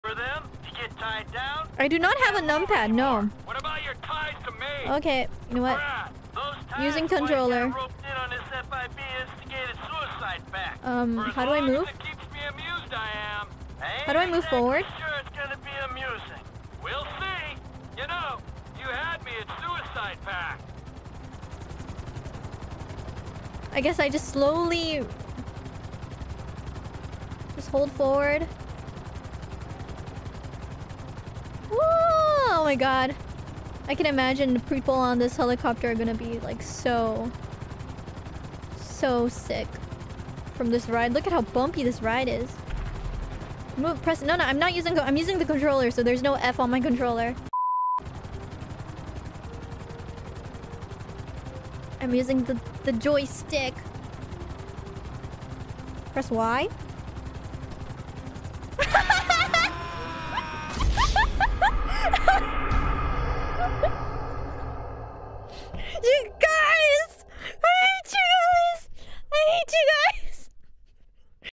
IRYS no cae en el viejo truco de "Presiona F" pero【hololive sub español】
lista de reproducción de más vídeos traducidos